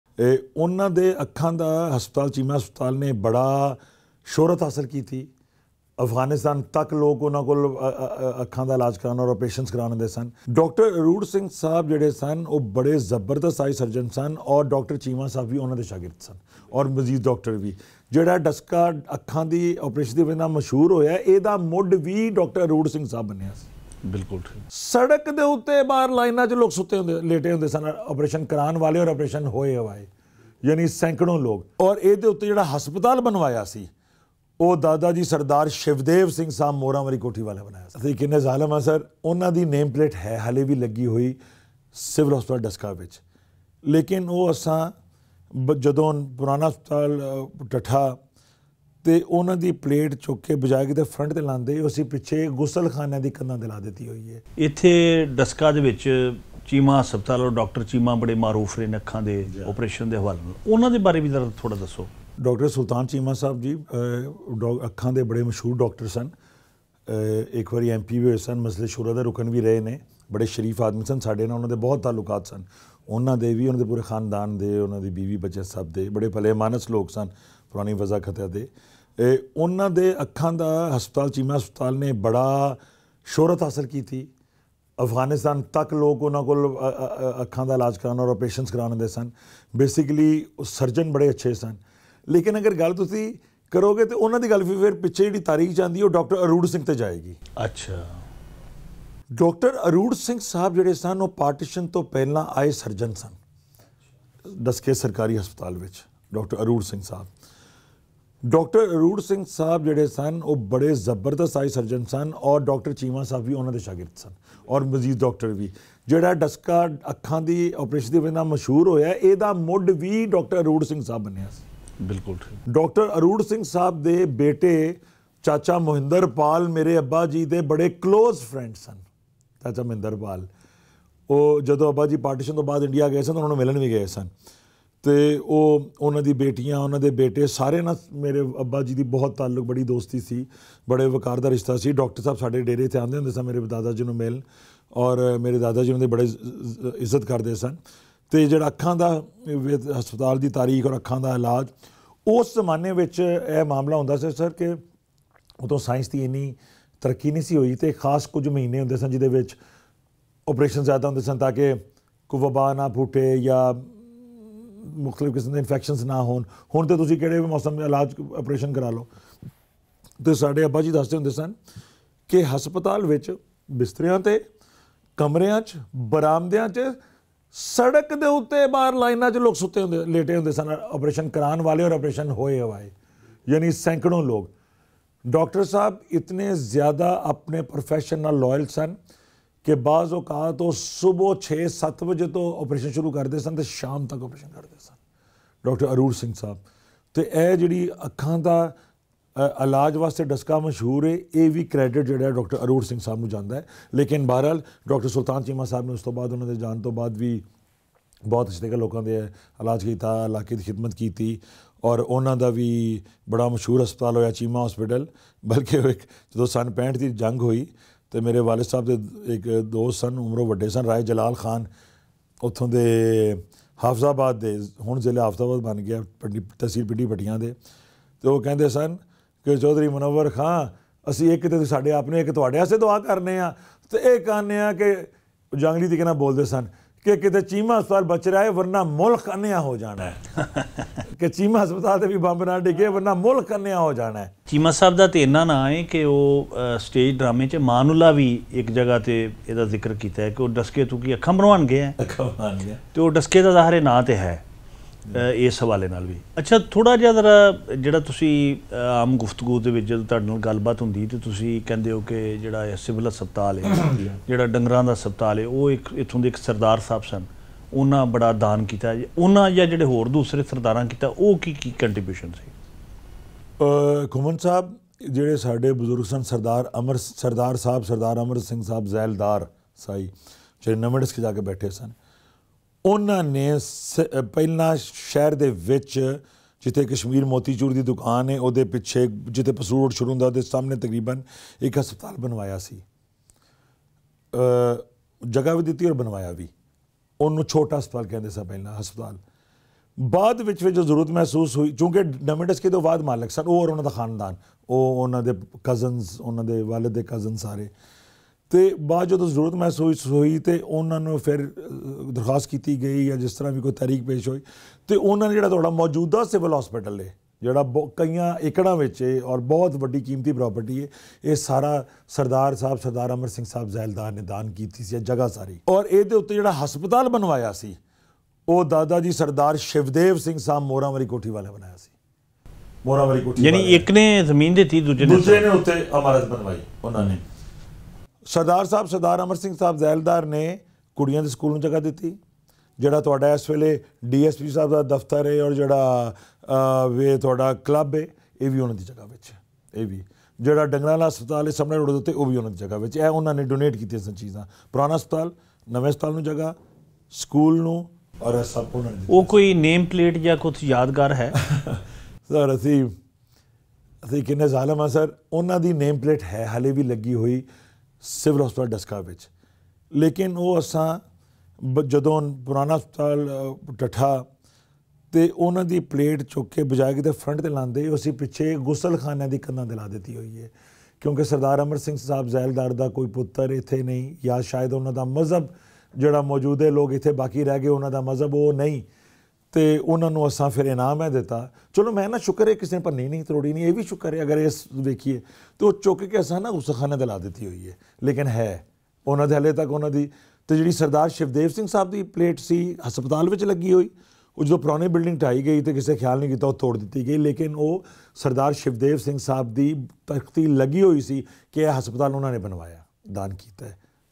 The Legacy of Cheema Hospital: A Journey Through History in Daska- ASG
Explore the fascinating history of Cheema Hospital, a cornerstone of healthcare in Daska. Discover how this renowned institution began, its evolution over the years, and its significant impact on the local community. Join us as we delve into the stories of the dedicated healthcare professionals who have contributed to its legacy and the milestones that have shaped its journey. Whether you're a local resident, a history enthusiast, or interested in healthcare developments, this video offers a comprehensive look into Cheema Hospital's past and its enduring influence in Daska.